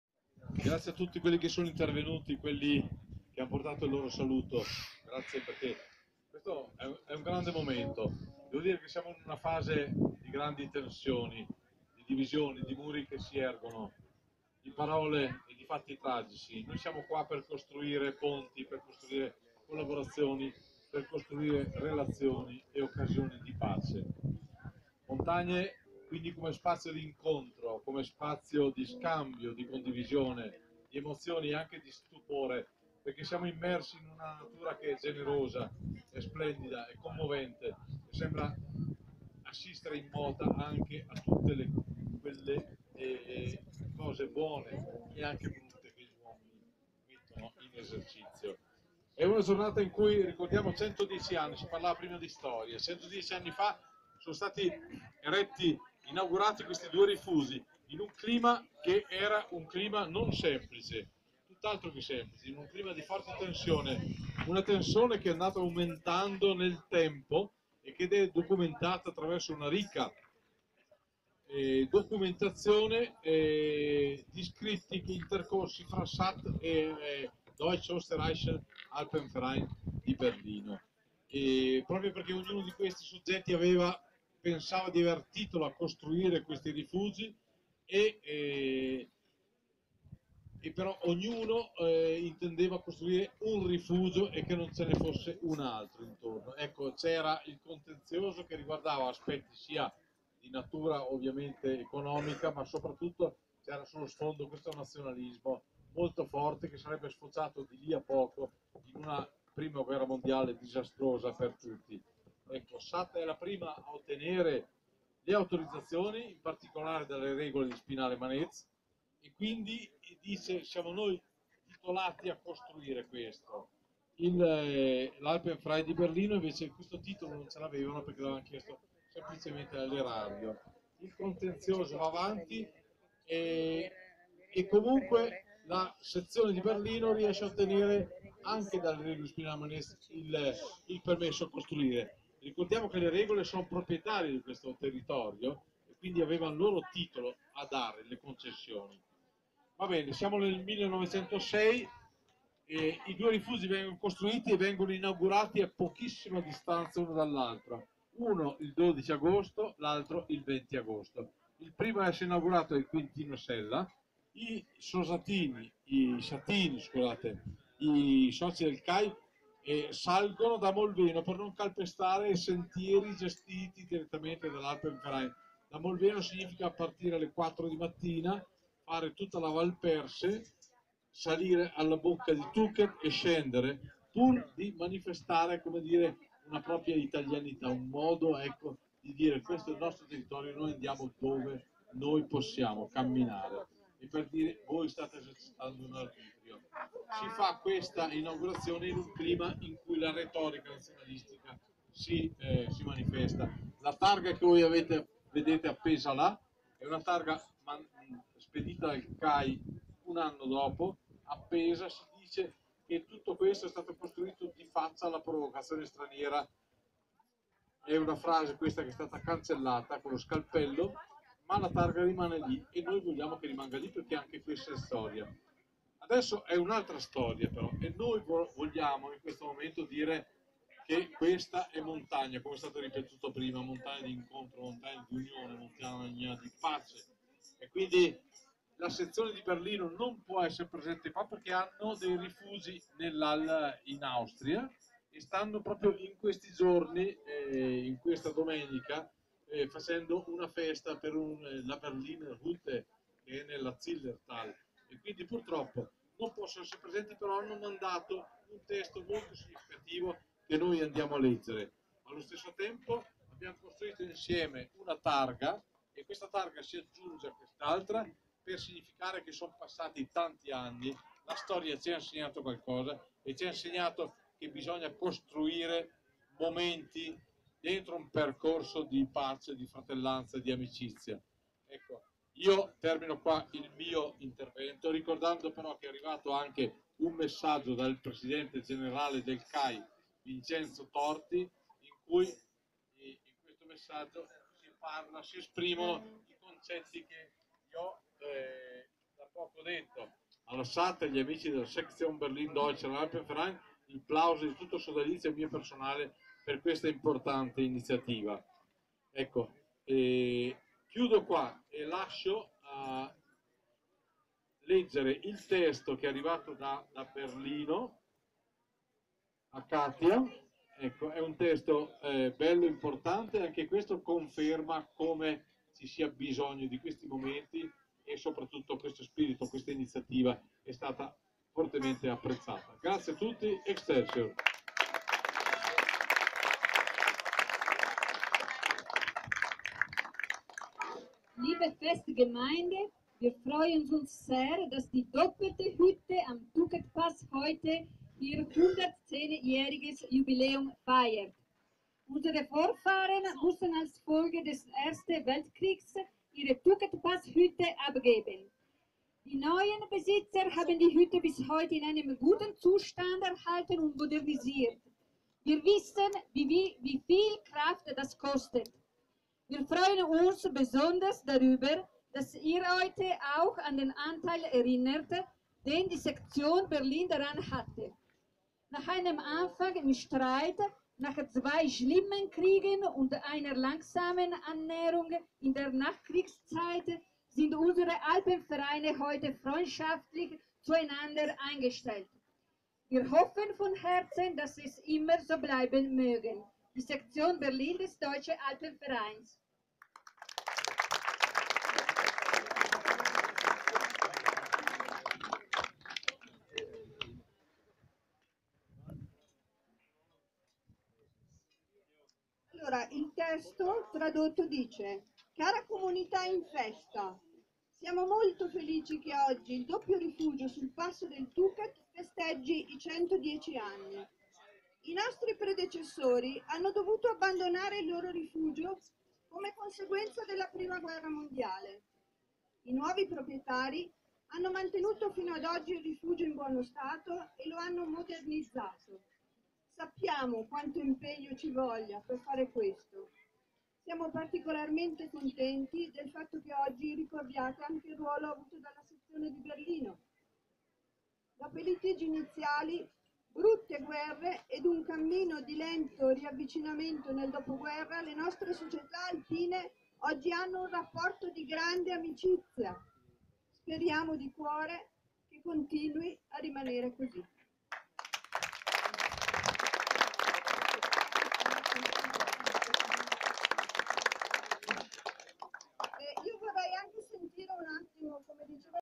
Pace fra Sat e Dav Berlin ai Rifugi Tuckett e Sella
Le celebrazioni per i 110 anni dei rifugi Francis Fox Tuckett e Quintino Sella nel Gruppo di Brenta in Trentino. Parla Claudio Bassetti, presidente della Sat del Trentino